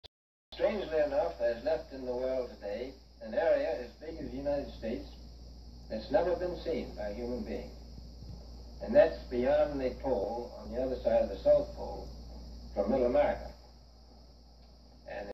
Admiral Byrd admits hidden land beyond Antarctica.
Small clip from the famous interview with Admiral Richard E. Byrd, admitting to the existence of land beyond the south pole.